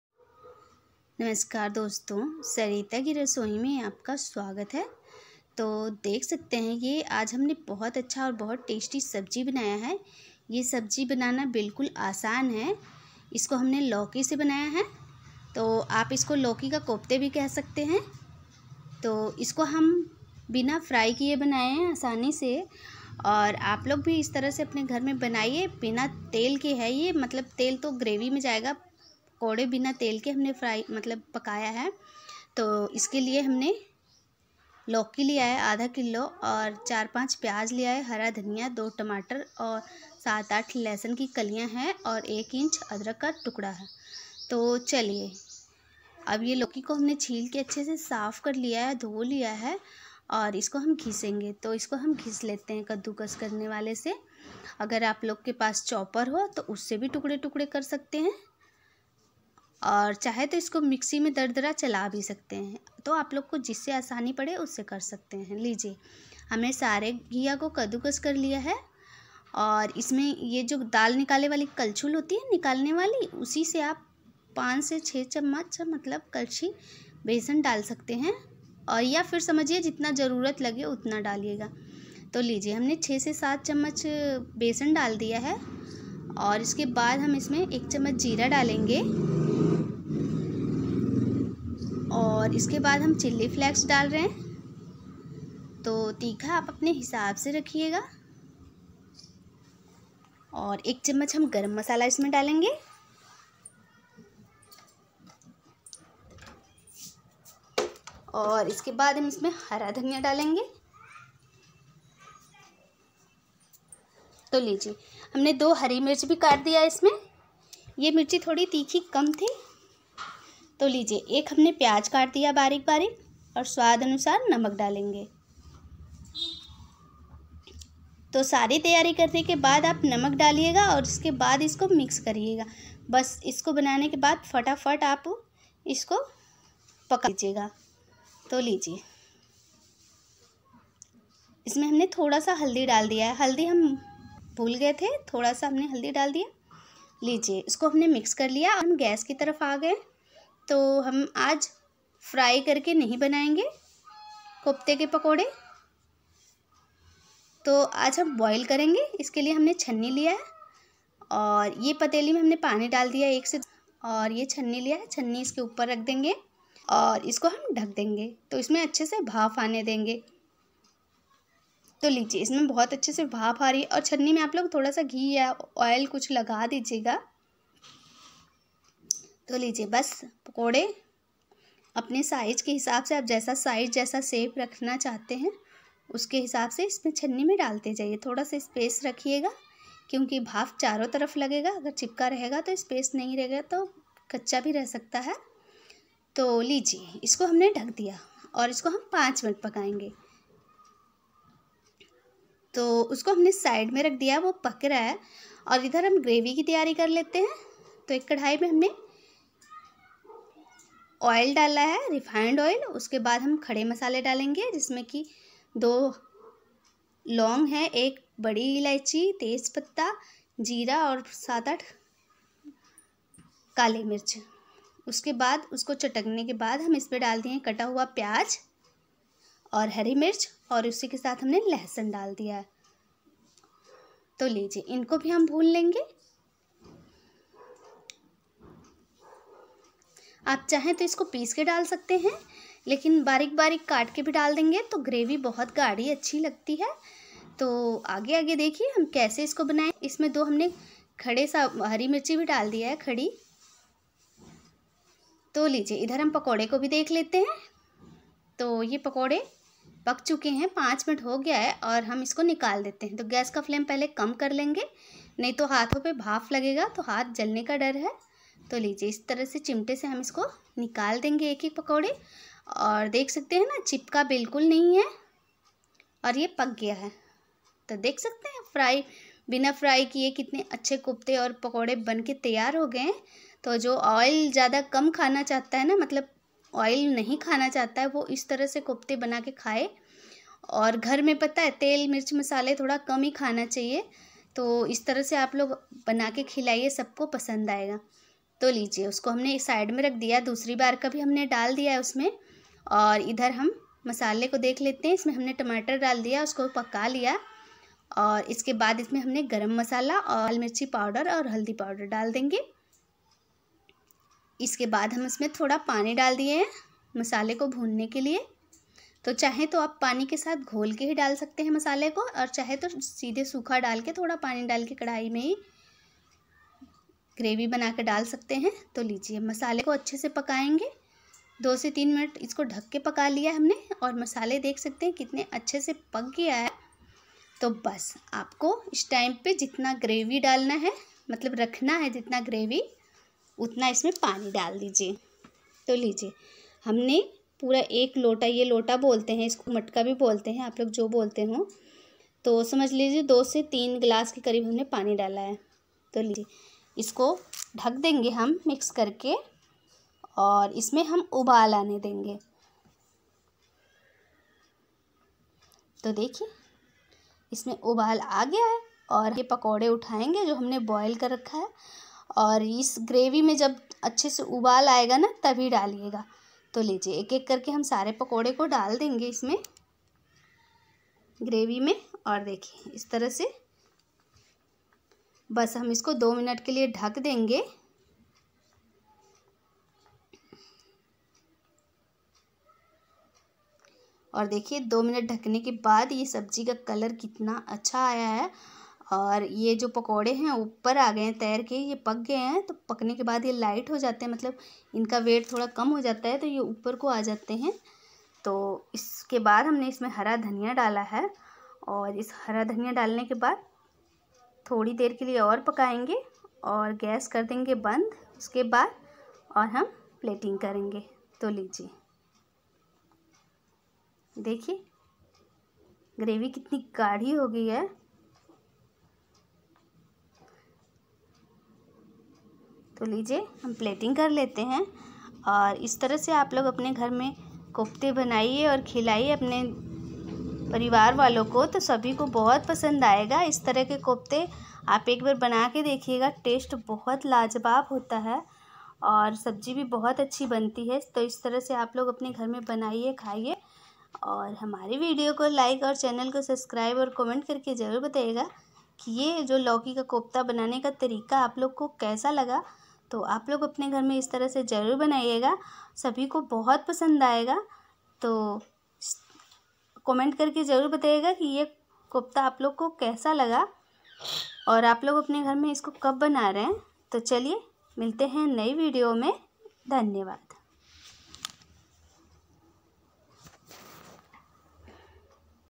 बिना तले बनाएँ बढिया लौकी के कोफ्ते एकदम नरम और चटपटे | Lauki k kofte recipe in Hindi
Hi friends 🖐

नमस्कार दोस्तों सरिता की रसोई में आज हमने लौकी के कोफ्ते बनाएँ हैं वो भी बिना तले अगर आपलोग भी बिना तले लौकी के कोफ्ते बनाना चाहते हैं तो इस वीडियो की मदद से आप आसानी से ये कोफ्ते बना सकते हैं और अगर आपको मेरी वीडियो अच्छी लगती है तो चैनल को सब्सक्राइब करें वीडियो को लाइक करें धन्यवाद

My some famous videos : - )